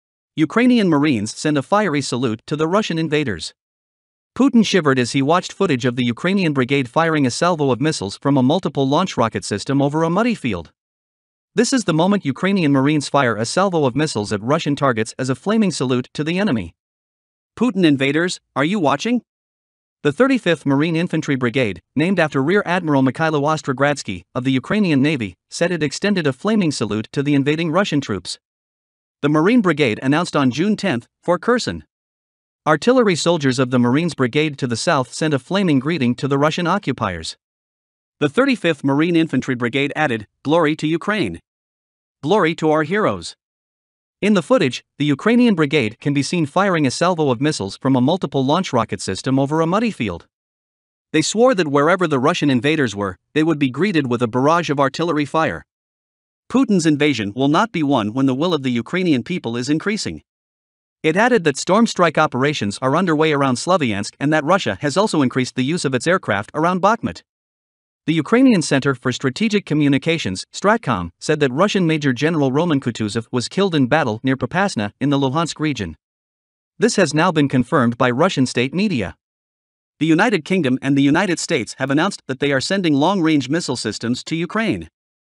Putin shivered as he watched Ukraine Marines Send Fiery Greeting To Russian Invaders
Ukrainian marines send a fiery salute to the Russian invaders.
Putin shivered as he watched footage of the Ukrainian brigade firing a salvo of missiles from a multiple launch rocket system over a muddy field.
This is the moment Ukrainian marines fire a salvo of missiles at Russian targets as a "flaming salute" to the enemy. Putin invaders, are you watching?

The 35th Marine Infantry Brigade, named after Rear Admiral Mykhailo Ostrogradsky, of the Ukrainian Navy, said it extended a "flaming salute" to the invading Russian troops.
The Marine Brigade announced on June 10: “For Kherson! Artillery soldiers of the Marines brigade to the south sent a 'flaming greeting' to the Russian occupiers.